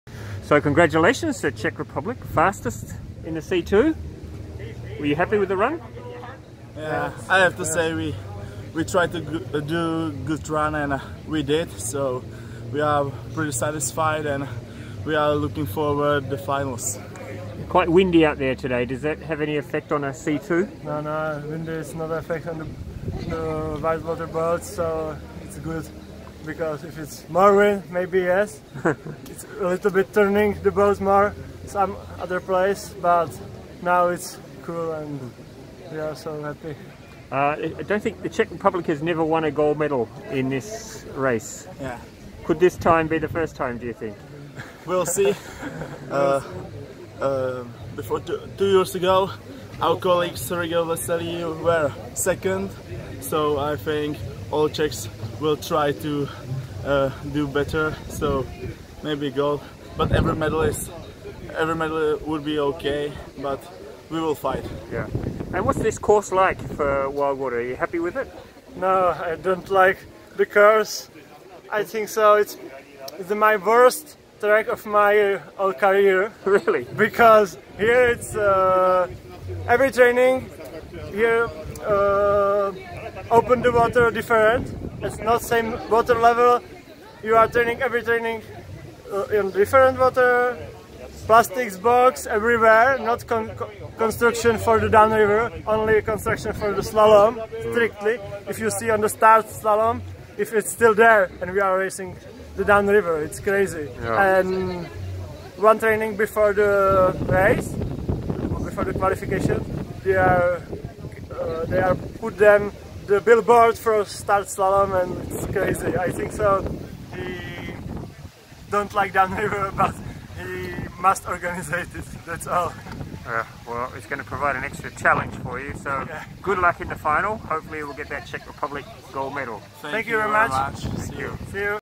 Czech C2 wildwater Suchanek and Rolenc
The Czech Republic has never won the men's C2 sprint at an ICF wildwater world championships. Daniel Suchanek and Ondrej Rolenc qualified fastest on Friday.